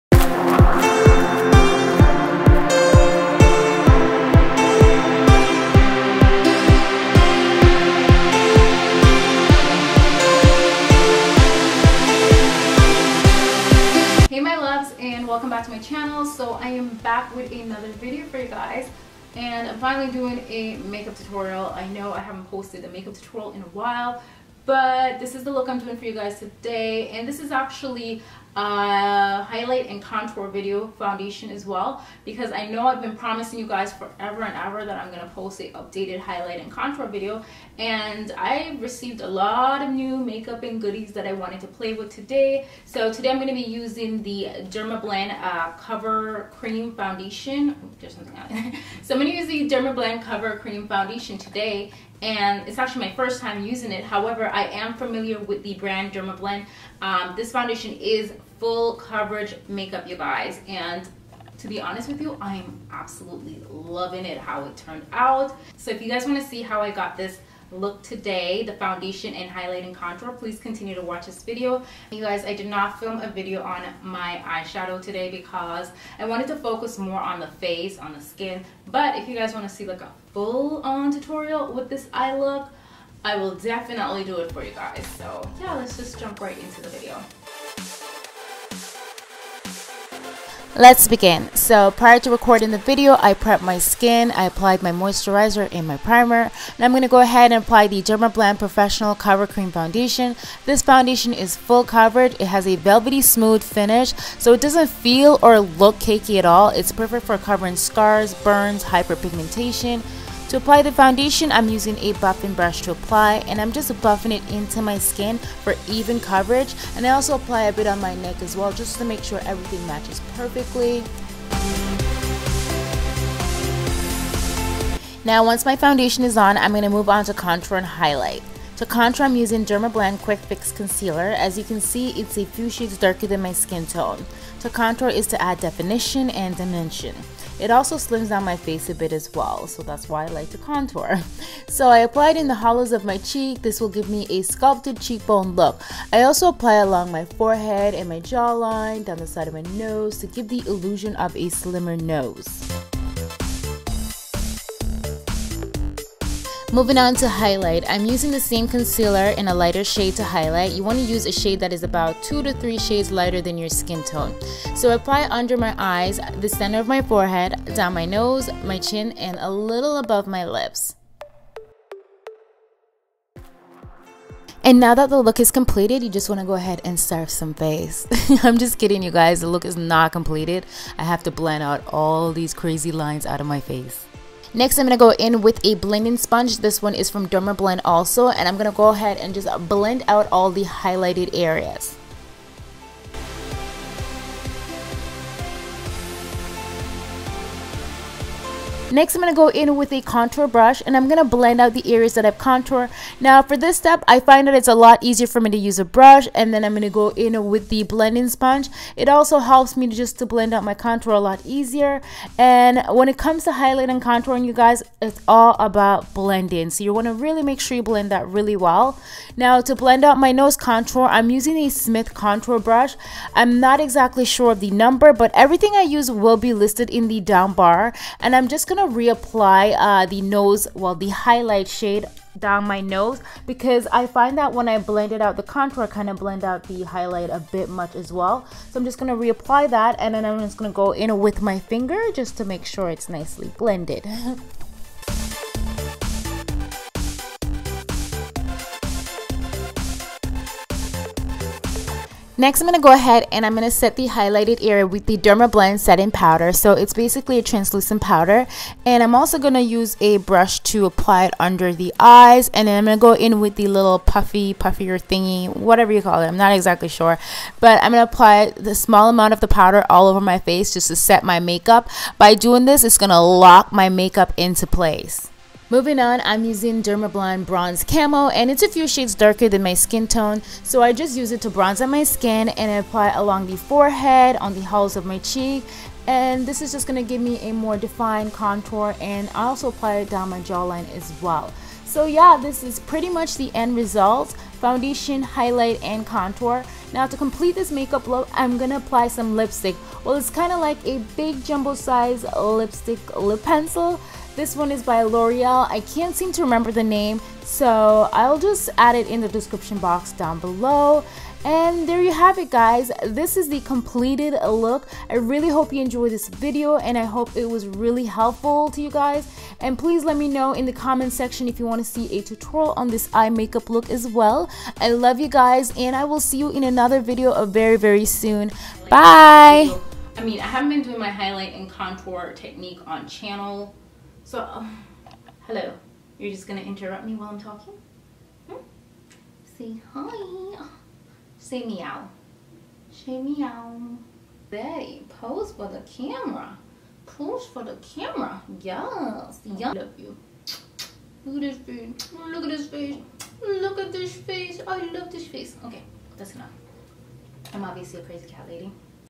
Foundation / Highlight & Contour / Dermablend
Hello My Loves , So this is a Full coverage Foundation routine Using Dermablend Cover Creme
 This foundation is perfect for covering blemishes , Hyper pigmentattion , Vitiligo  , Bruise , Burns

 Dreamland  Cover Cream Foundation  - Olive brown
Dermablend Setting Powder - Original
Quick Fix concealer  full coverage Concealer Dermablend - caramel
Intense Powder Camo - Bronze - Dermablend - Deep